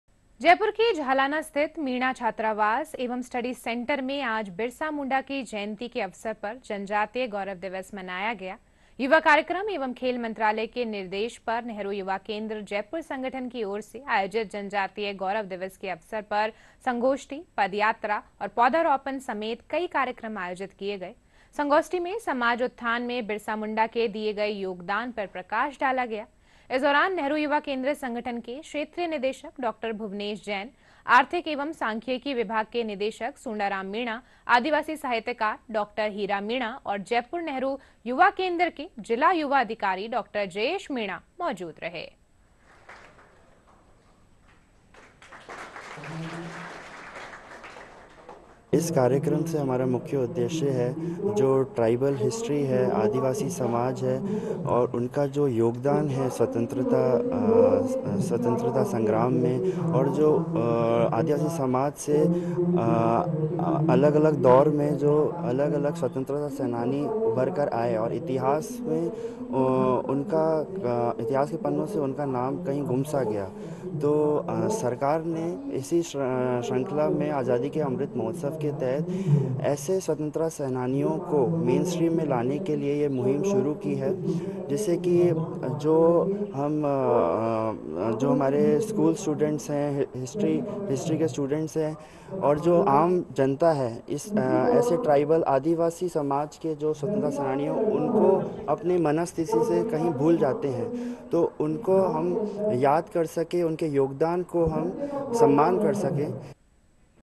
Jaipur : बिरसा मुंडा की जयंती पर जयपुर के मीणा छात्रावास में जनजातीय गौरव दिवस कार्यक्रम का आयोजन
Jaipur : बिरसा मुंडा की जयंती पर जयपुर के मीणा छात्रावास में जनजातीय गौरव दिवस कार्यक्रम का आयोजन ... नेहरू युवा केंद्र की ओर से आयोजित कार्यक्रम में संगोष्ठी, पदयात्रा और पौधारोपण समेत विभिन्न कार्यक्रमों का किया गया आयोजन